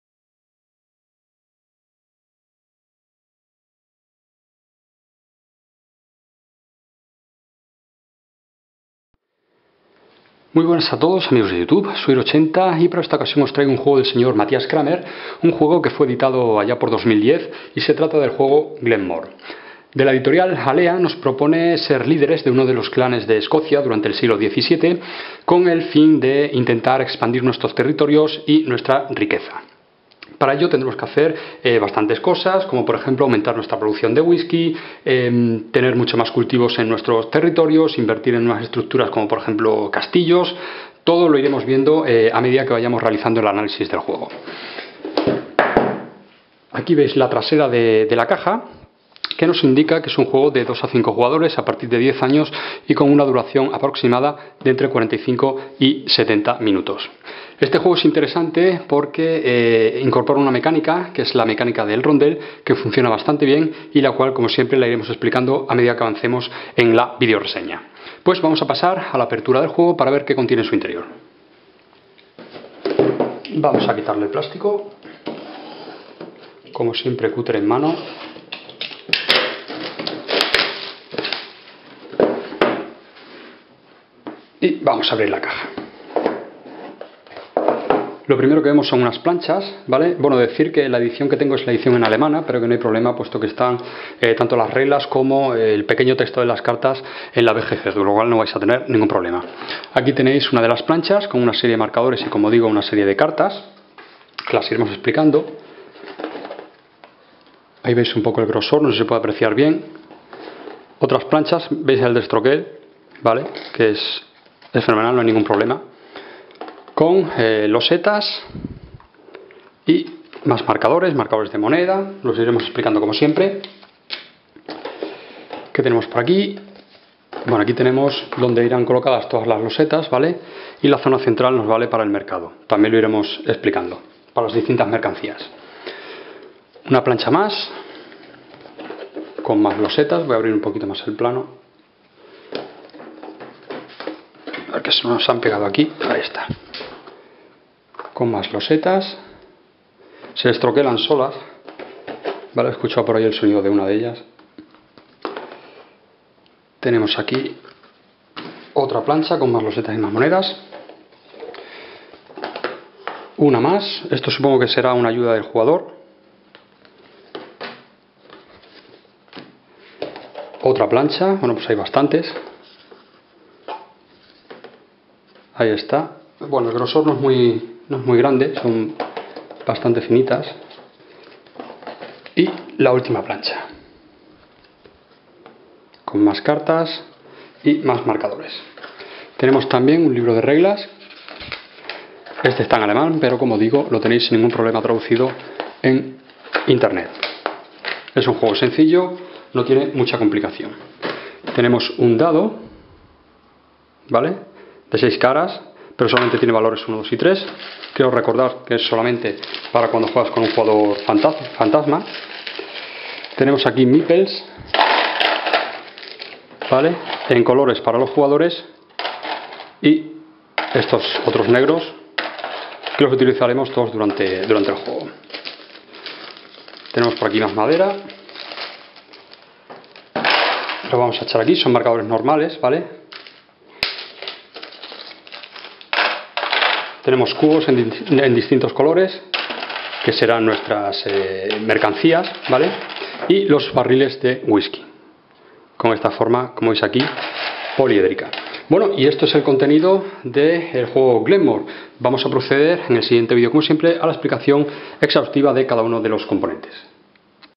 Glen More - Intro & unboxing
Comenzamos la vídeo reseña/análisis de "Glen More", este juego de Cramer de colocación de losetas y recursos. Para empezar, una pequeña "intro, y el "unboxing" del mismo.

Saludos!!!